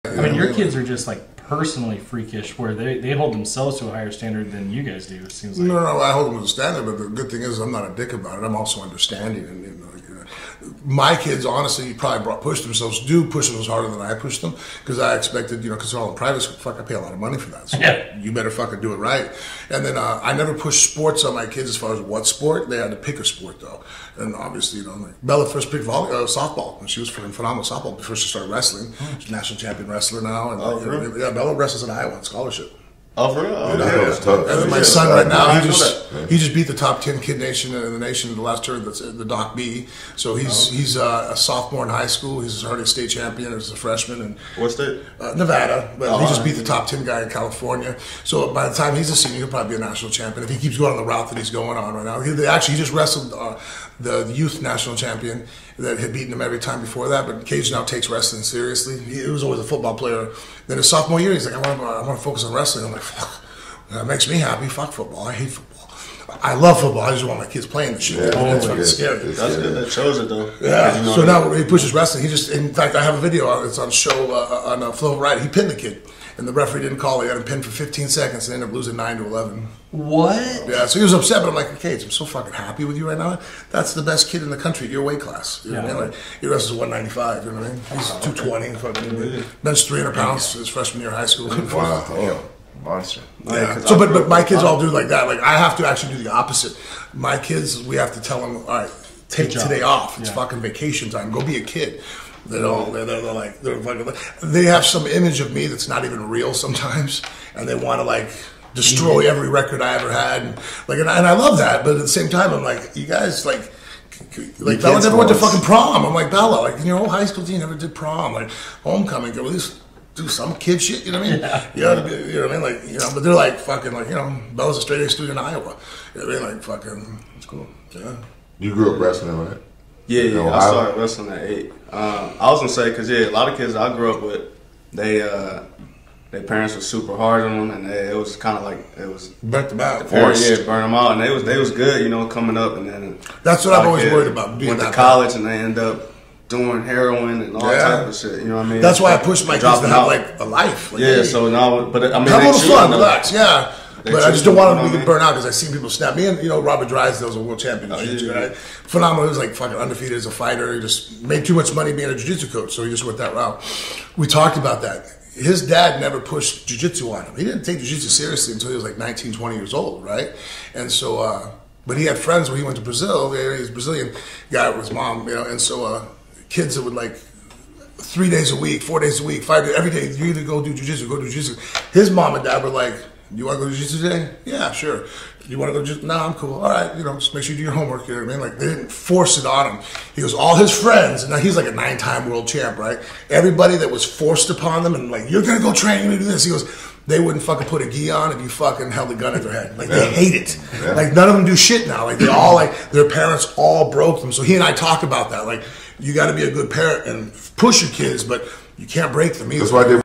MMA Legend Frank Mir Gives INCREDIBLE Advice for Parents | The HJR Experiment
Hear the full story on Episode 10 of The HJR Podcast